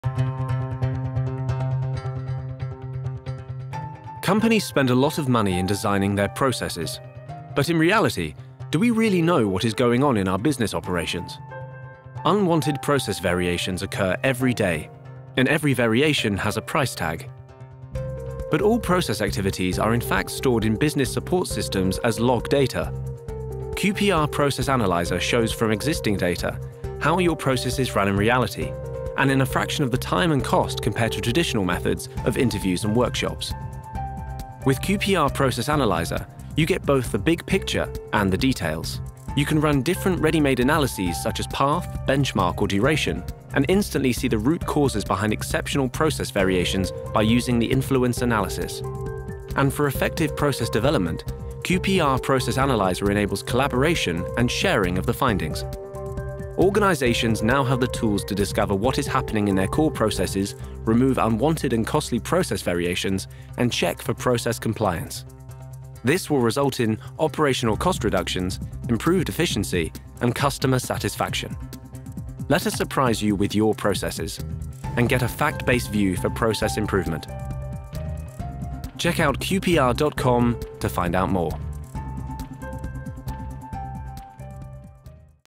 QPR ProcessAnalyzer - Process Mining in a Nutshell
Companies spend a lot of money in designing their processes. But in reality, do we really know what is going on in our business operations? Unwanted process variations occur every day, and every variation has a price tag. QPR ProcessAnalyzer shows you how your processes run in reality, discovers from YOUR data the accurate and fact based reality of your operations.